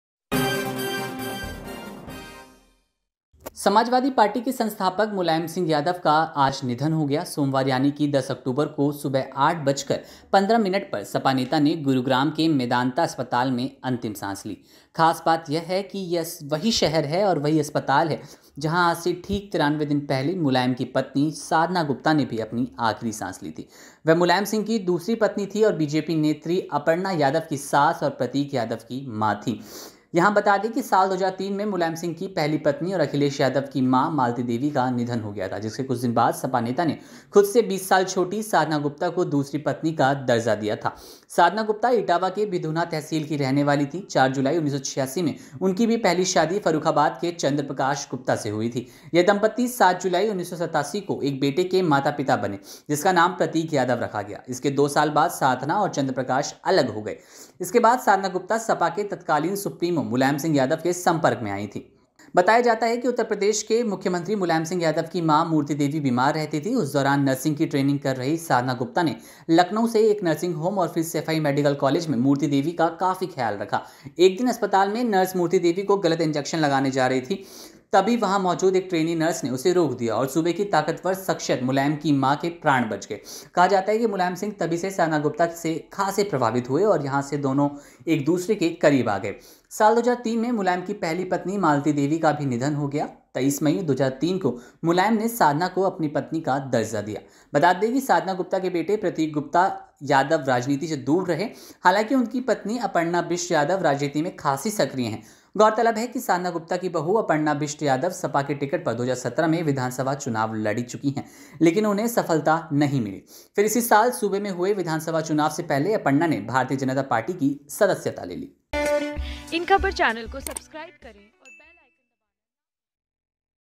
Mulayam Singh Yadav Net Worth: इतनी संपत्ति छोड़ गए मुलायम सिंह यादव, लिया था कर्ज | Mulyam Death
Mulayam Singh Yadav Net Worth: साल 2019 में संपन्न हुए लोकसभा चुनाव के दौरान दाखिल किए गए हलफनामे के मुताबिक, मुलायम सिंह यादव की कुल नेटवर्थ 16,52,44,300 करोड़ रुपये थी. इसके साथ ही इसमें बताया गया था कि सपा संरक्षक के पास खुद की कोई कार नहीं थी.